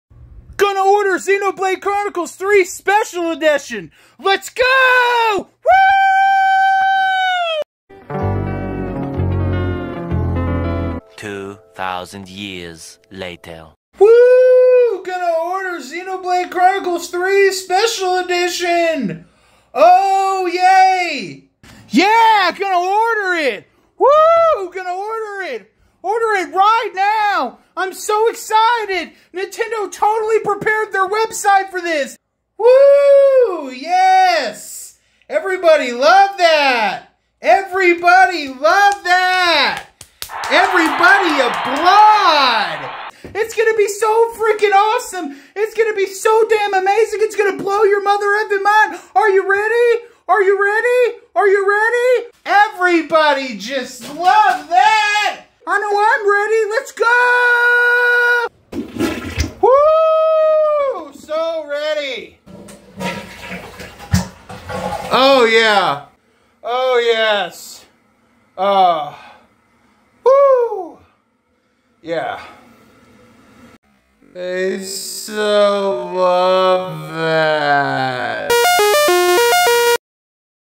Everyone's experience ordering Xenoblade Chronicles 3 Special Edition
I know everybody is really frustrated that Nintendo once again drops the ball and is not prepared for the heavy traffic when it comes to a highly sought after collectors edition. Or when they don't make enough to meet demand. I decided to make a funny little video and light of this situation cuz we all need to laugh and blow off some steam. If you like this video give me some love by following my channel sharing my videos and following me on Twitter love you all. Link to the original Twitter post below.